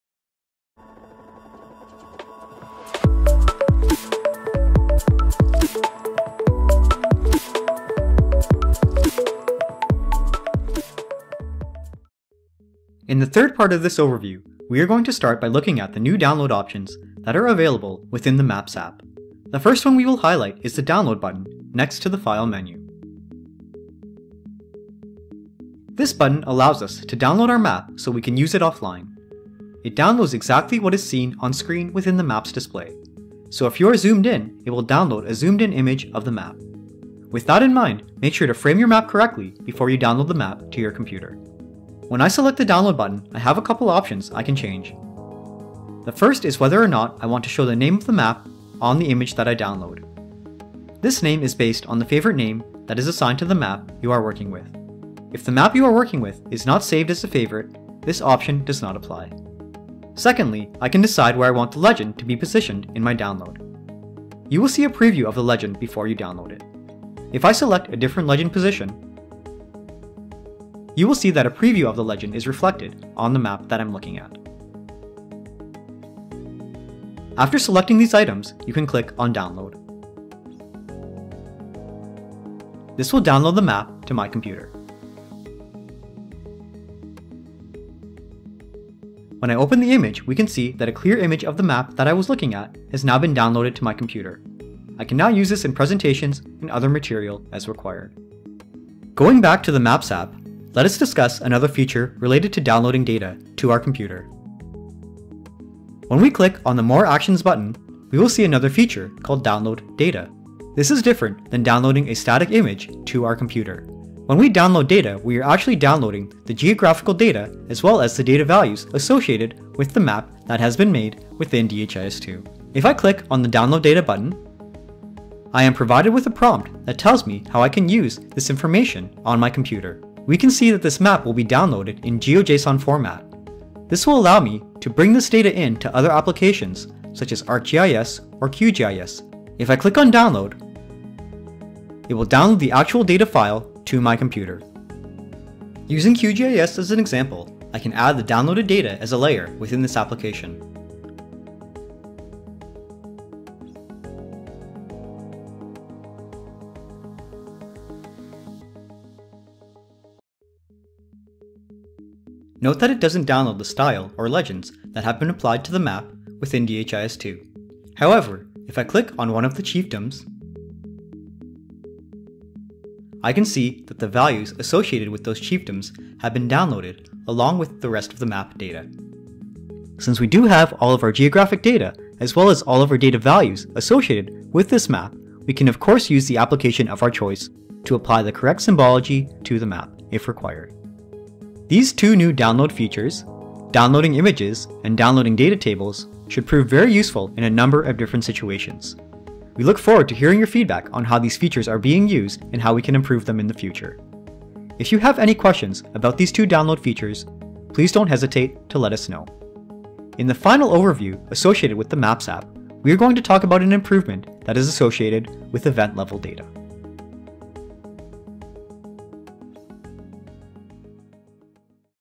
DHIS 2.31 Maps App - Part 3 - Downloading Map Data
This video provides an overview of the re-designed maps app in DHIS 2.31. The maps app replaces the GIS app. This overview is separated into 4 parts. In part 3, new download features are discussed.

All of these features can be reviewed in the online DHIS2 demo